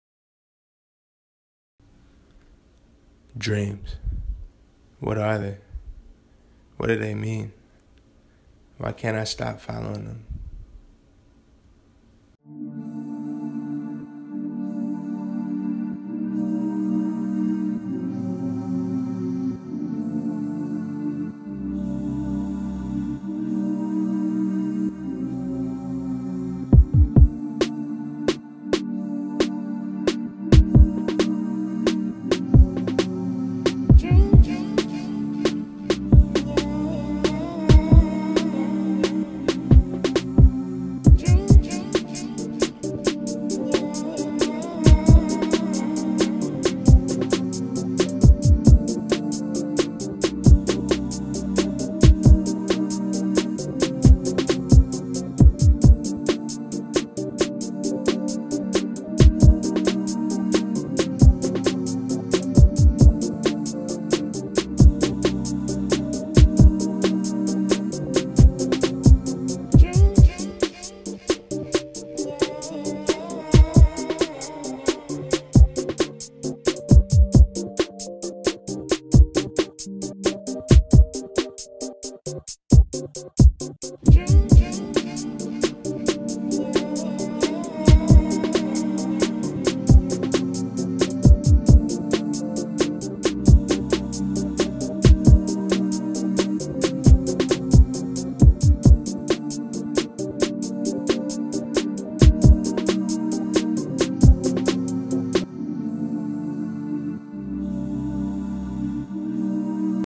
Dreams + Never Give Up (Vlog 5)
Decided to switch the Vlog up again today. I want to remind everyone out there that there is purpose in your Journey, never lose sight of what you've been put here on this Earth to do. Hope you enjoy the visuals and the beat I made for the Vlog! Thanks so much for the support!